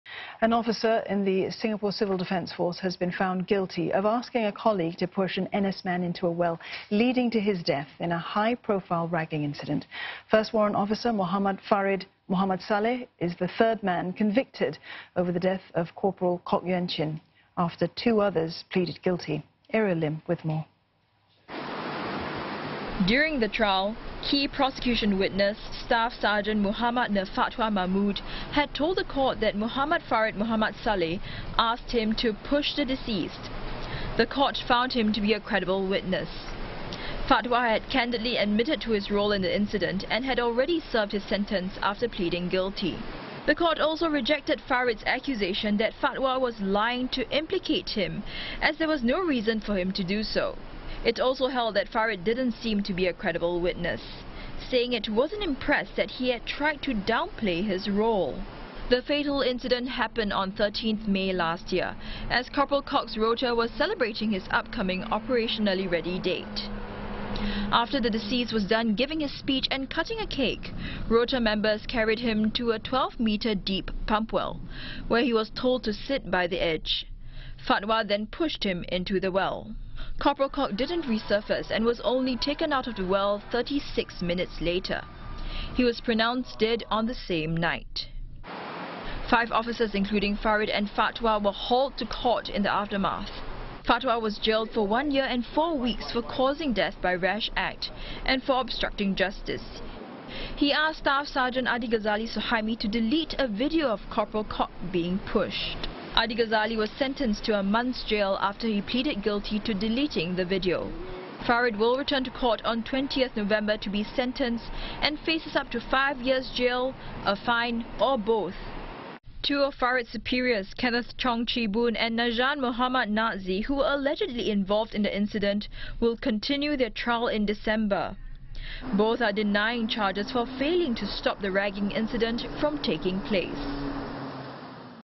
SCDF ragging trial: Officer found guilty of telling colleague to push NSF into pump well
A Singapore Civil Defence Force officer has been found guilty of asking a colleague to
push an NSF into a well leading to his death in a high-profile ragging incident. First
Warrant Officer Mohamed Farid Mohd Saleh was found guilty of instigating his colleague,
Staff Sergeant Nur Fatwa Mahmood, to push Corporal Kok Yuen Chin into a pump well
at Tuas View Fire Station. Farid is the third man convicted over the death of Corporal Kok.